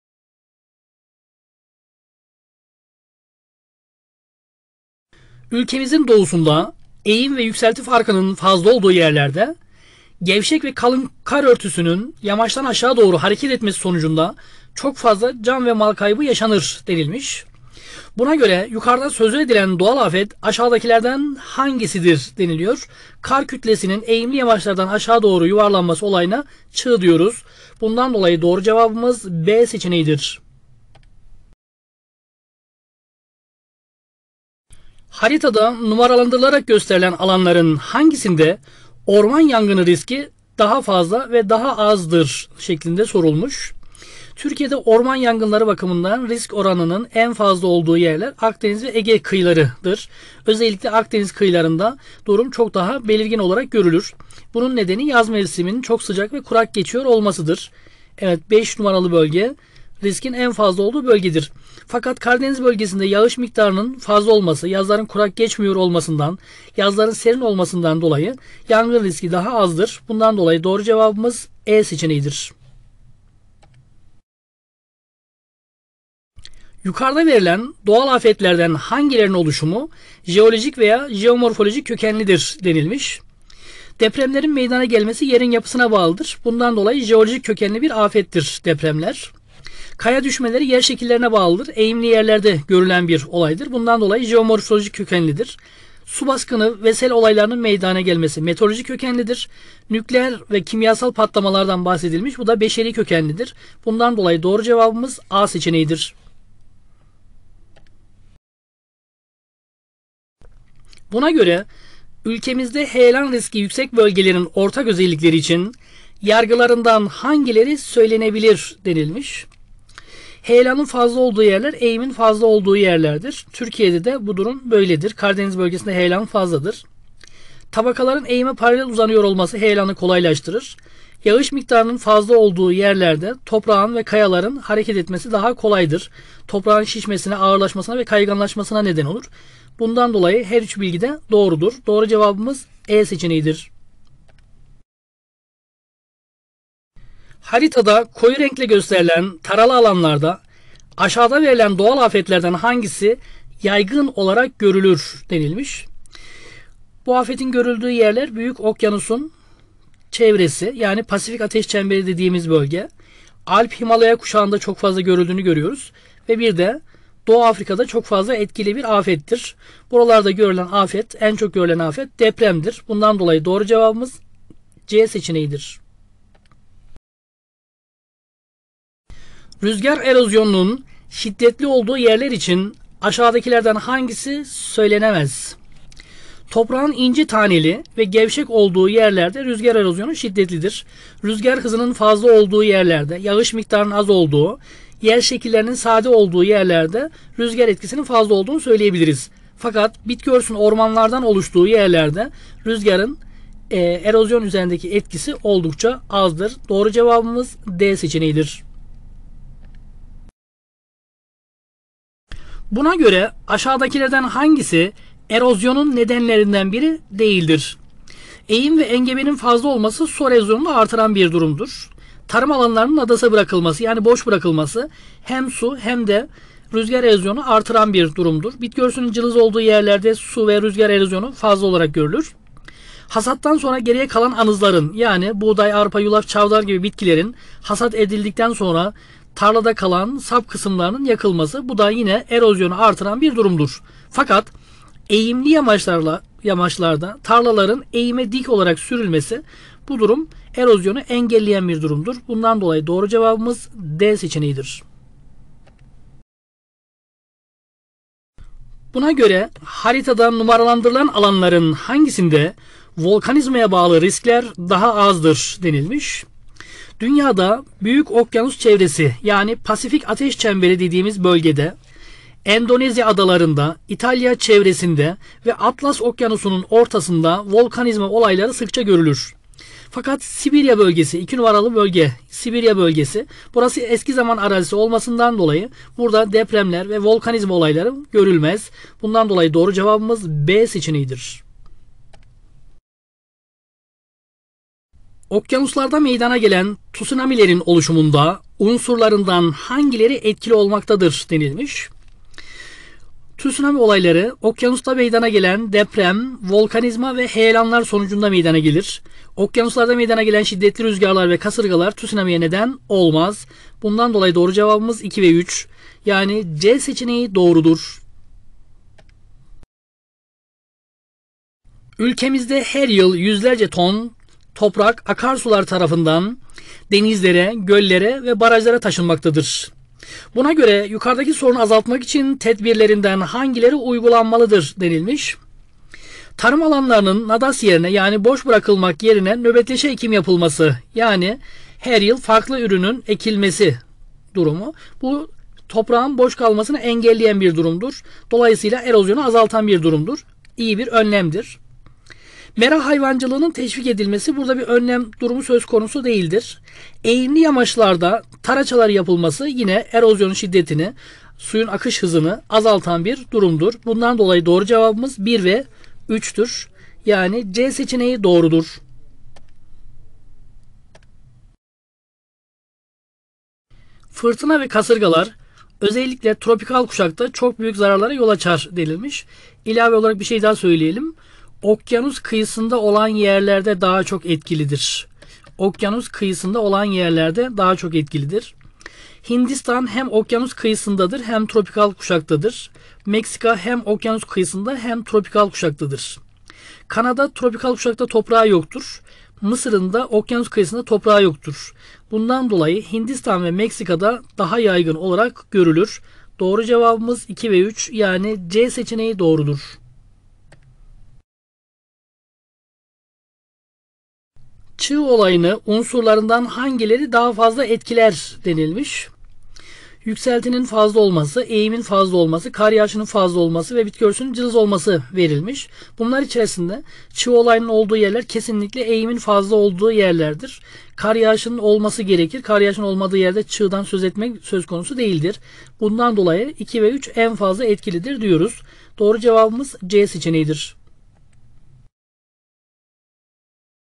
TYT Coğrafya Soru Bankası Bölüm 17 Test 2 Soru Çözümleri
Soru 01 = 00:00:03
Soru 02 = 00:00:34
Soru 03 = 00:01:15
Soru 04 = 00:01:49
Soru 05 = 00:02:26
Soru 06 = 00:02:58
Soru 07 = 00:03:31
Soru 08 = 00:04:17
Soru 09 = 00:04:54
Soru 10 = 00:05:21
Soru 11 = 00:06:13
Soru 12 = 00:06:52